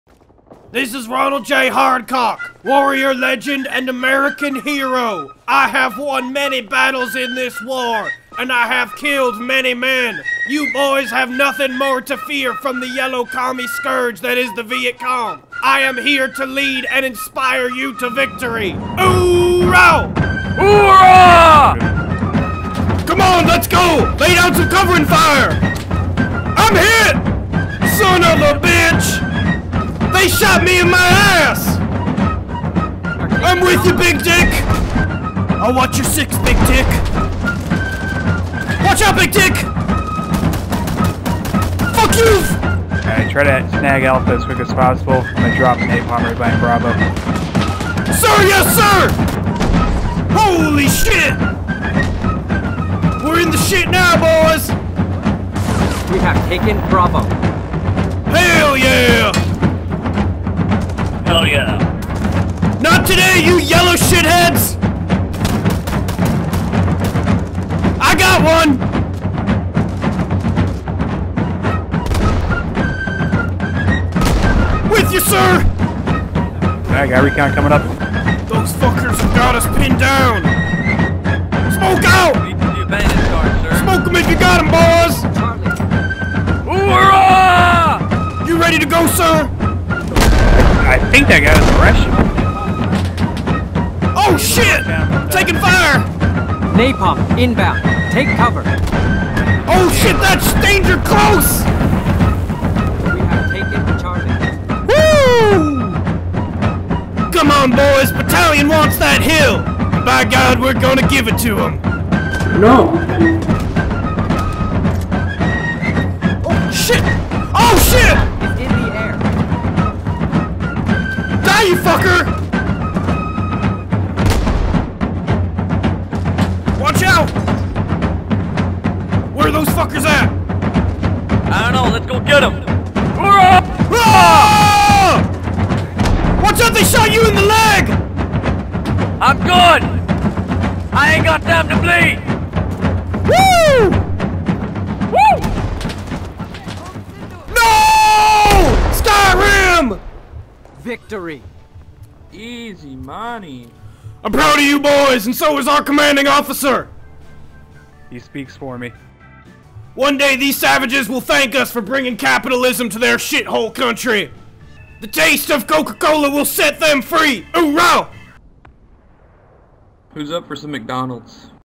Ronald J. Hardcock Inspires His Men to Achieve Victory
RONALD J. HARDCOCK, WARRIOR LEGEND AND FREEDOM FIGHTER, LEADS HIS MEN TO A GALLANT VICTORY THROUGH HIS HEROIC INSPIRATION AND FEARLESS LEADERSHIP, AND KILLS ANY MOTHERFUCKER WHO TRIES TO STAND IN HIS WAY